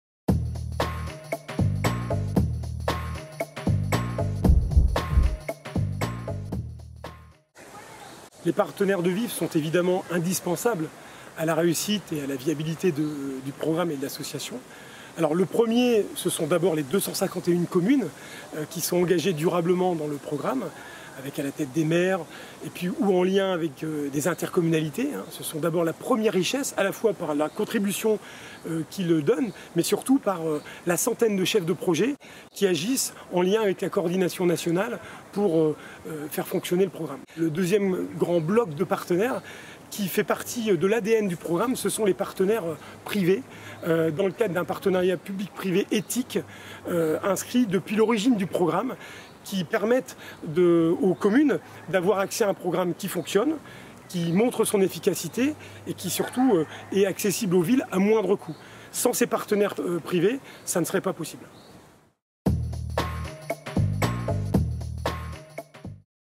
Au Coeur de VIF #51 - Quels sont les partenaires VIF ? - Thibault DESCHAMPS
51ème capsule vidéo de la saga "Au coeur de VIF" !

VIF, programme national de promotion de la santé et de la forme, donne la parole aux acteurs qui permettent de le faire exister et rayonner.

Zoom sur les partenaires publics et privés, piliers du programme VIF®, avec Thibault DESCHAMPS, président de l’association :

✅ Plus de 250 villes et EPCI, des élus et chefs de projet qui déploient les actions de prévention santé VIF sur le terrain
✅ Des partenaires privés engagés, dans le cadre d’une charte éthique, qui permettent le déploiement du programme à grande échelle, en offrant aux territoires l’accès à une méthodologie efficace à moindre coût

En somme : un partenariat public-privé éthique, où chaque acteur fait sa part pour la mise en oeuvre d'actions efficaces, dans la proximité et sur la durée... pour des populations en forme !